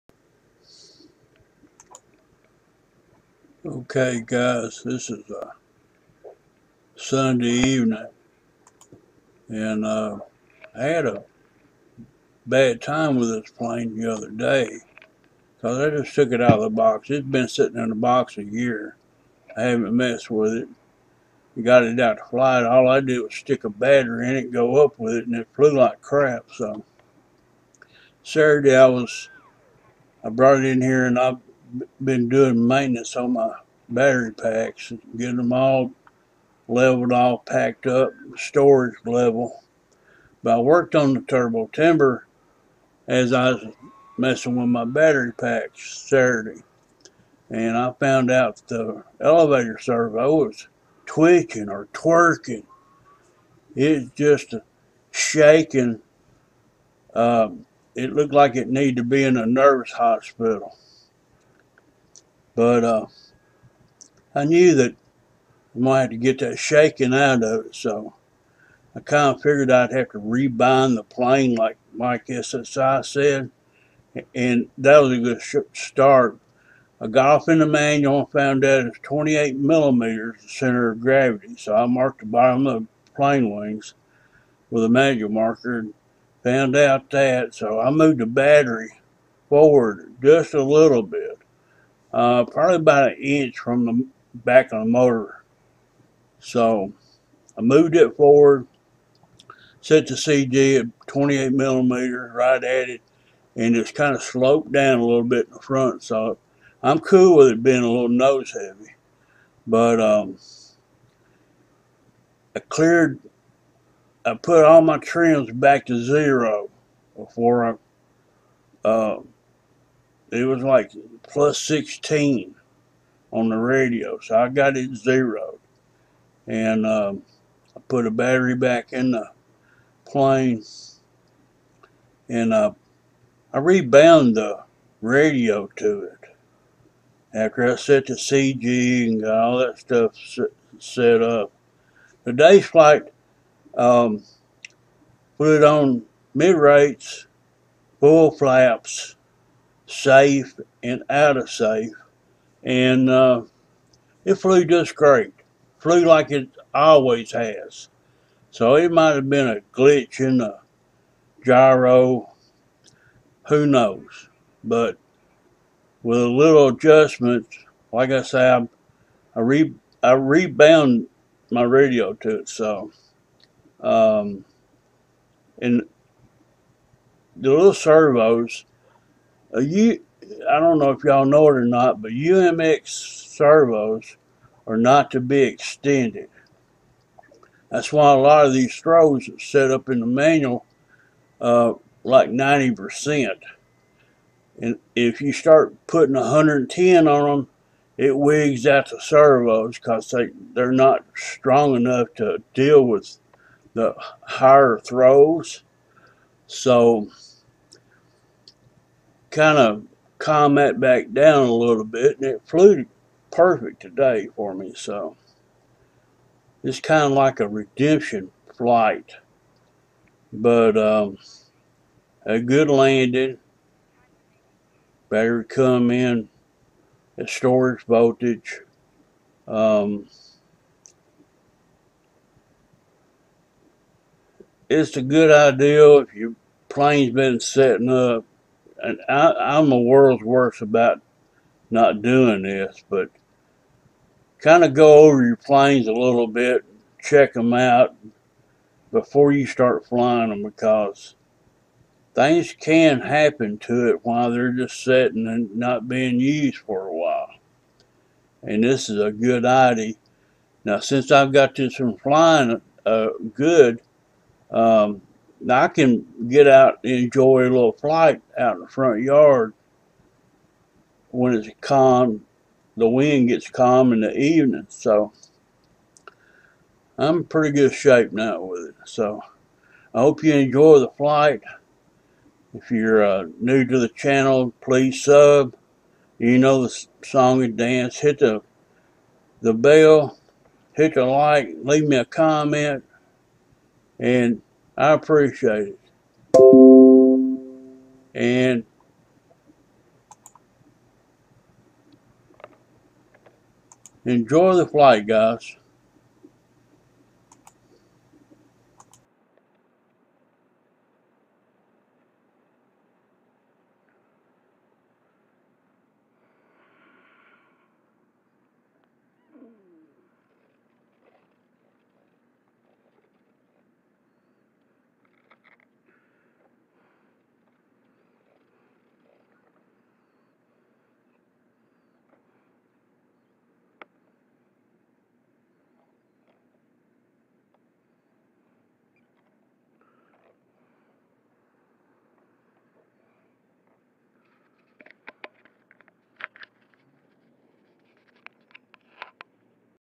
I love this plane UMX Turbo Timber 5/15/2022 Re-Maiden flight, no trim needed
plane check - re-did cg to 28mm, move battery forward about a inch from back of motor (little nose heavy), remove all trim from radio from the other flight ,(crap show) rebind plane. found elevator twitching, need to be in the nervous Hospital LOL

camera runcam2 1080p 60fps
video editing: VideoPad Professional Ver. 7.02  NCH Software